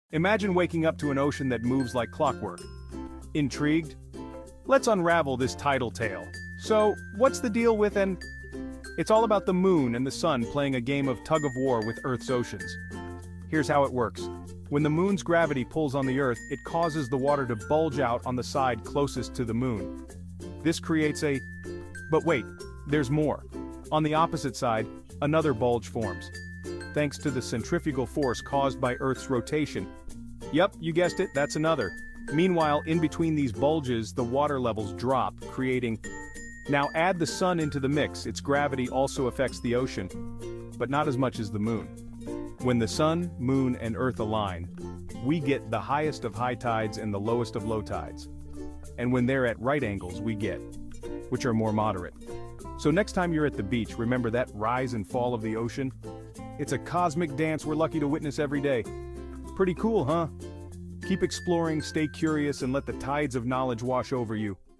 High Tides vs Low Tides: Understanding the Science Behind Ocean Movements
Dive into the fascinating world of oceanography as we explore the differences between high tides and low tides! In this informative video, we break down the science behind these natural phenomena, revealing how the gravitational pull of the Moon and the Sun influences the rise and fall of ocean waters.